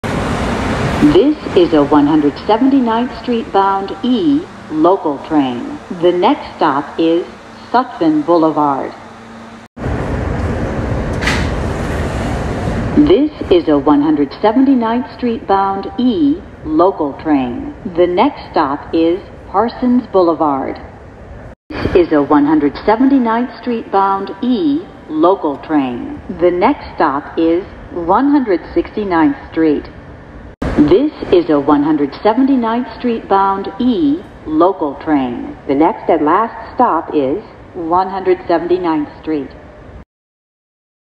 R160 E Train Announcements To Jamaica-179 Street (Pm Rush Hour)
Rush hour E to 179 St announcements